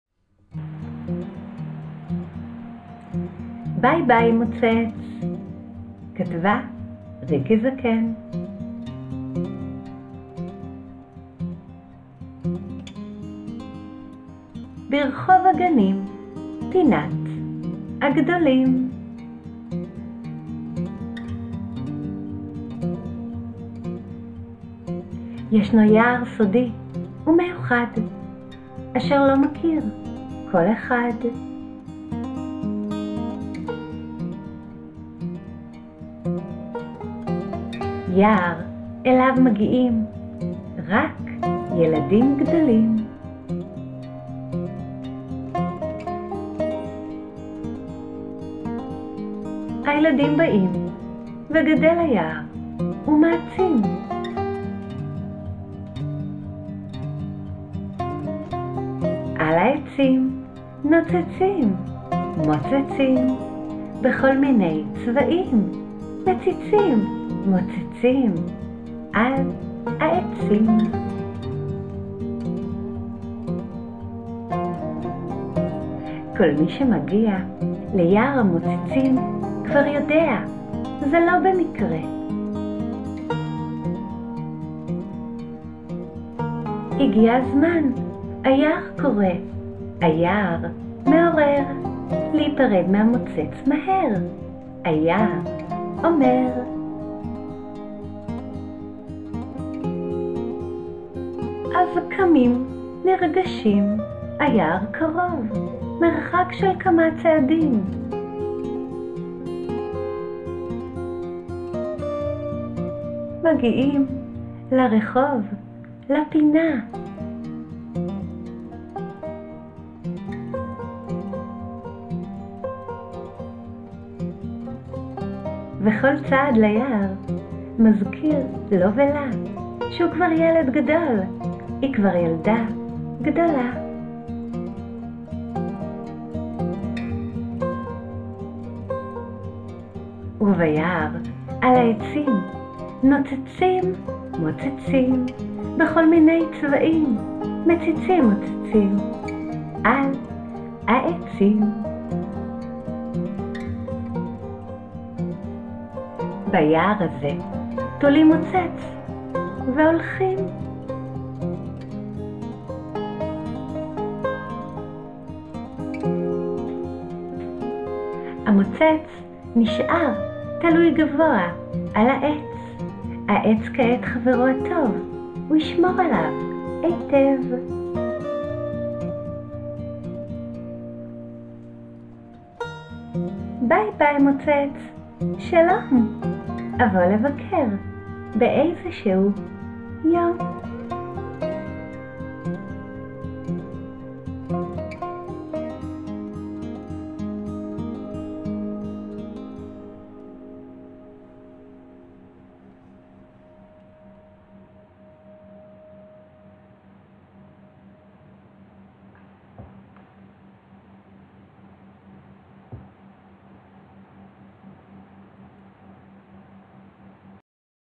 ביי ביי מוצץ - סיפור קצר לילדים - כתבה: ריקי זקן
ביי ביי מוצץ
סיפור  פרידה מהמוצץ לילדים